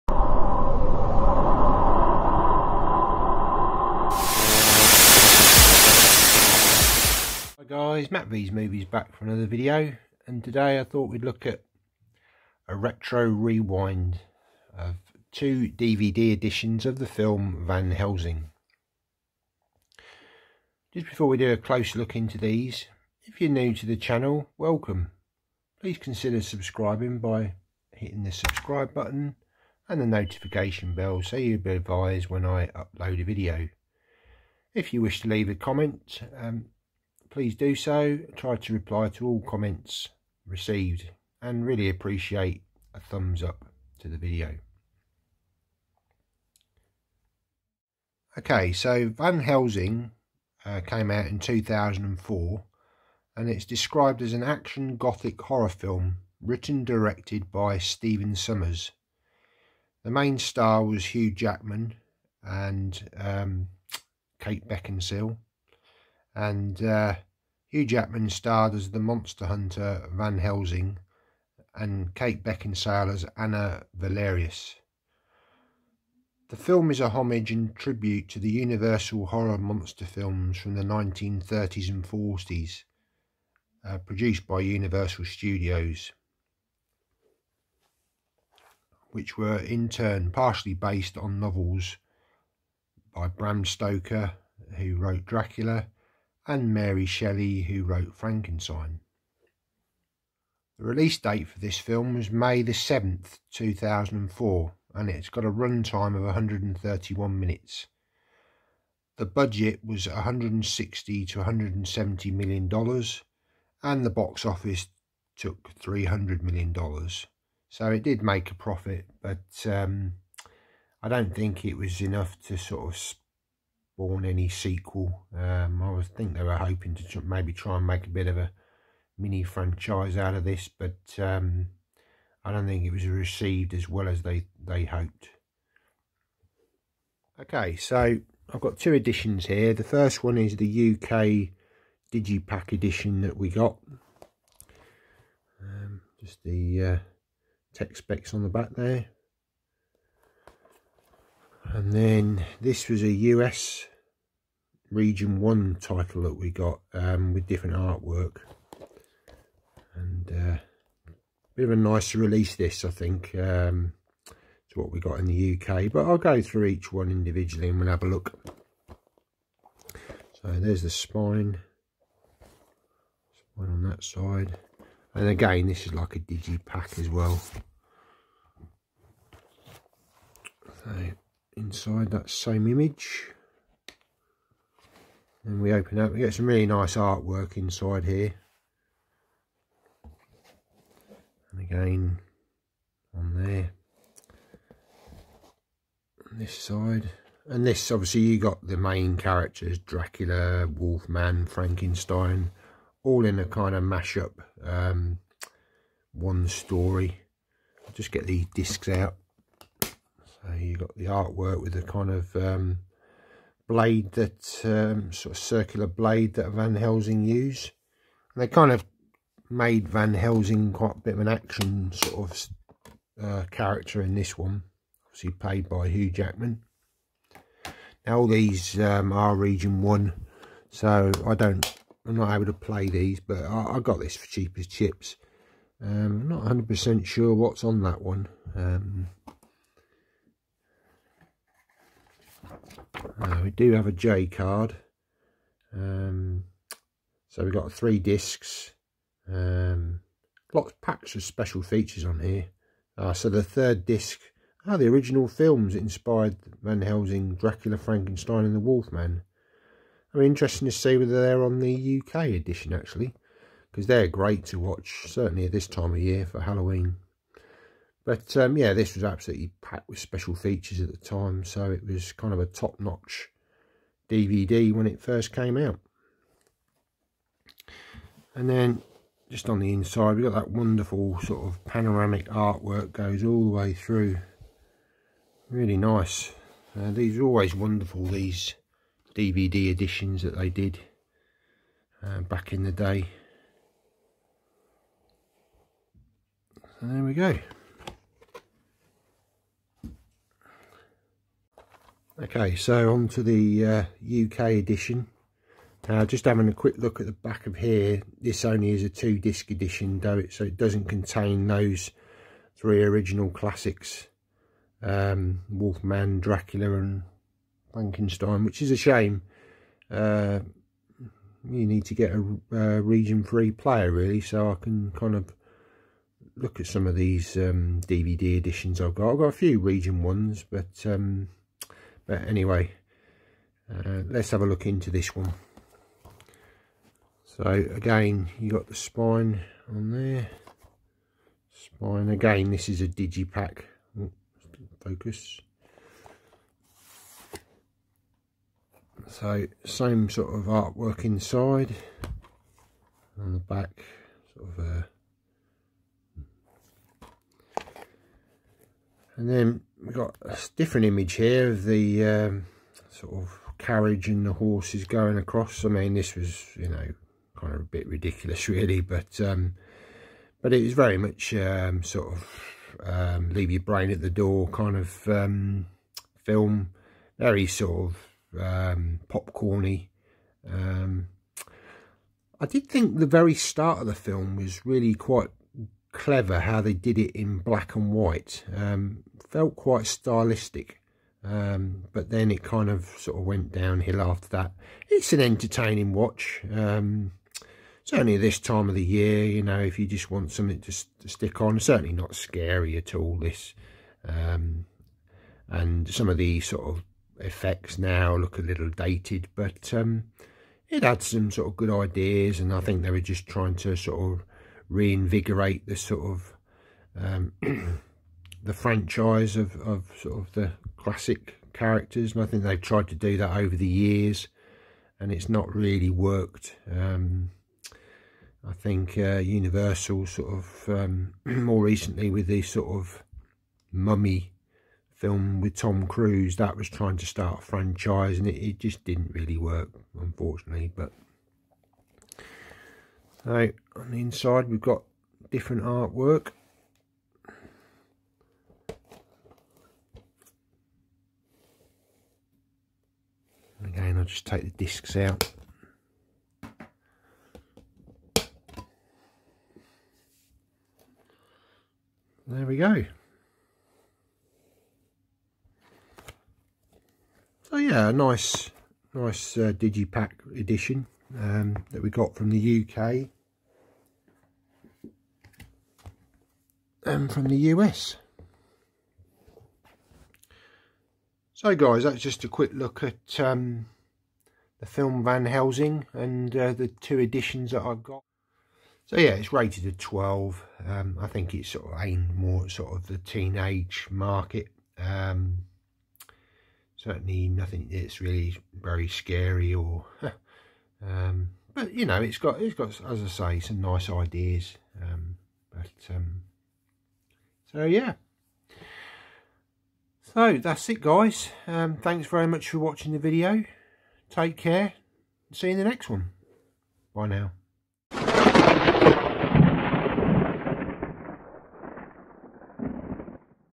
RETRO REWIND ~ A CLOSER LOOK AT VAN HESLING DVD EDITIONS 🎃
Here I show my two  DVD editions of the film Van Hesling.